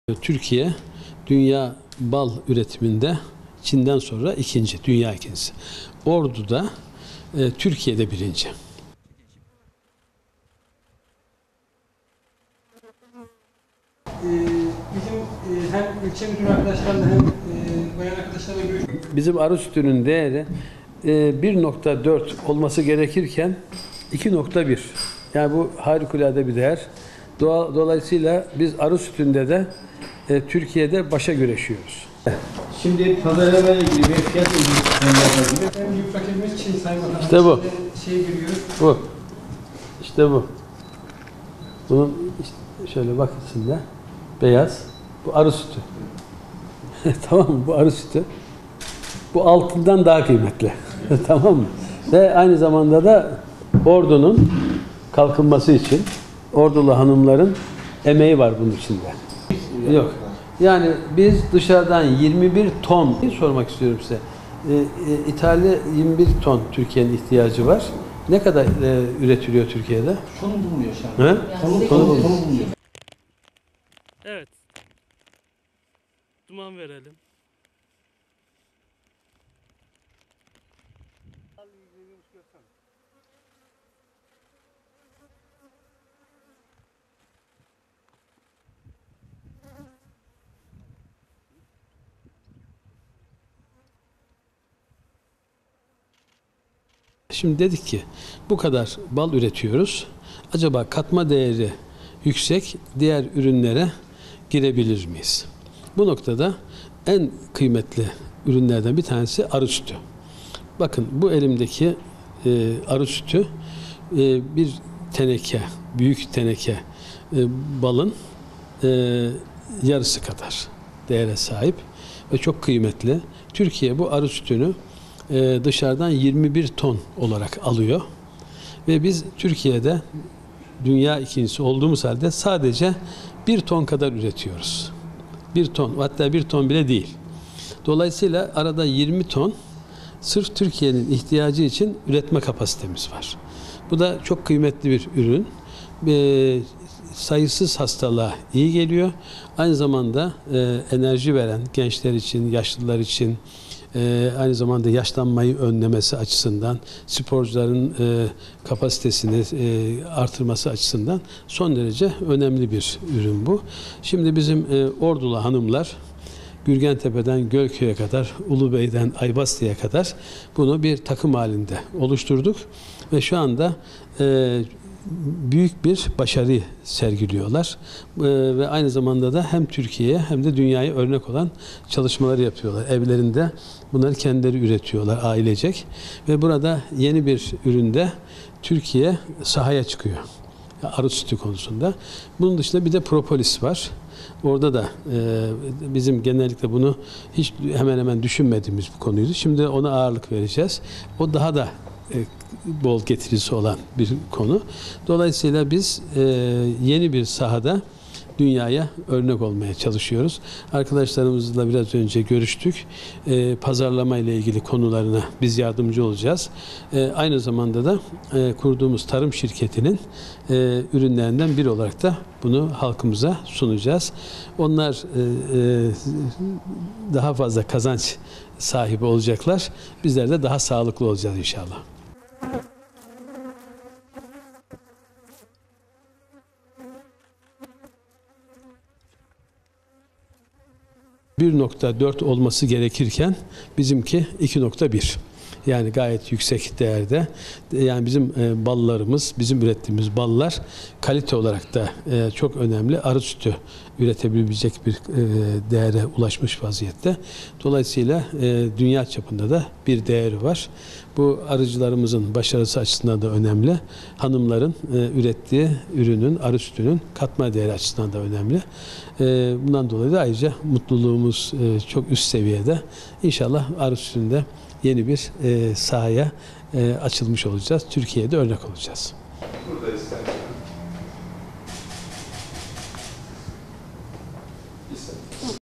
Arı Sütü Kadın Çiftçilerin Yeni Gelir Kapısı Oldu
Ordu Büyükşehir Belediye Başkanı Dr. Mehmet Hilmi Güler, Ordu Büyükşehir Belediyesi ile Arıcılık Araştırma Enstitüsü Müdürlüğünün ortaklaşa yürüttüğü Önder Kadın Çiftçiler Yetiştirmeye Yönelik Arı Sütü Eğitimi Projesi kapsamında kadın çiftçiler ile bir araya geldi.
Dünyanın en pahalı sütleri arasında yer alan arı sütü üretimini teşvik etmek amacıyla Ordu Büyükşehir Belediyesi ile Arıcılık Araştırma Enstitüsü Müdürlüğü tarafından ‘Önder Kadın Çiftçiler Yetiştirmeye Yönelik Arı Sütü Eğitimi Projesi’ kapsamında mayıs ayından itibaren kadın çiftçilere yönelik eğitim süreci başlatıldı. Kadın çiftçiler, verilen eğitim sonrası kilosu kalitesine göre 5-7 bin lira arasında değişen arı sütünü üretmek için Ordu’nun çeşitli ilçelerinde çalışmalara başladı. Yaklaşık 5 aylık süre zarfında çalışmalarını sürdüren kadın çiftçiler, yapılan analizler sonucunda arı sütünün fiyatının belirlenmesinde temel kalite parametresi olan ve en az 1,4 değere sahip olması gereken ‘10 Hidroksi Dekonoik Asit’ (HDA) değerinde 2,1 yüzdesiyle karşılaşarak bir başarıya imza attılar.
Kadın çiftçileri ağırlayan Ordu Büyükşehir Belediye Başkanı Dr. Mehmet Hilmi Güler, çiftçilerin üretim aşamasında yaşadıkları zorlukları dinleyerek arı sütünü önemli bir gelir kaynağı haline getirmek için çalışmalarını sürdüreceklerini söyledi.